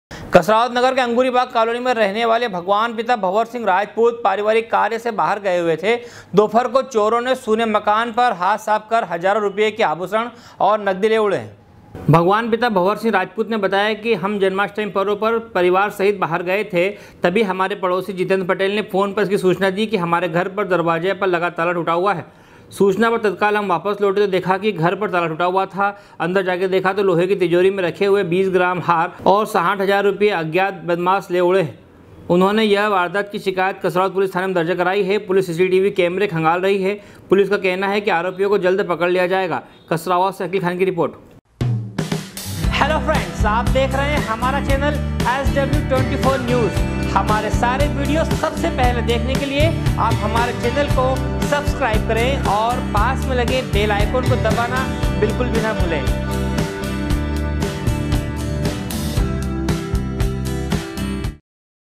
कसरावद नगर के अंगूरी बाग कॉलोनी में चोरों ने सुने मकान पर हाथ साफ कर आभूषण और नकदी ले उड़े।
कसरावद नगर के अंगूरी बाग कॉलोनी में रहने वाले भगवान पिता भंवर सिंह राजपूत पारिवारिक कार्य से बाहर गए हुए थे दोपहर को चोरों ने सुने मकान पर हाथ साफ कर हजारों रुपए के आभूषण और नकदी ले उड़े। कसरावद से अकिल खान की रिर्पोट।